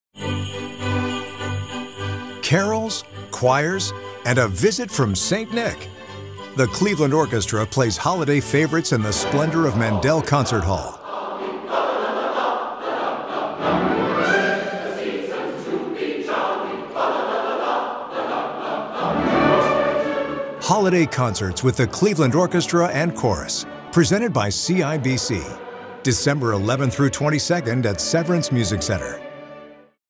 Holiday Concerts 2024
Holiday Concerts with The Cleveland Orchestra and Cleveland Orchestra Chorus returns in 2024! Join us for an unforgettable experience that will fill Mandel Concert Hall with the sounds of the holiday season.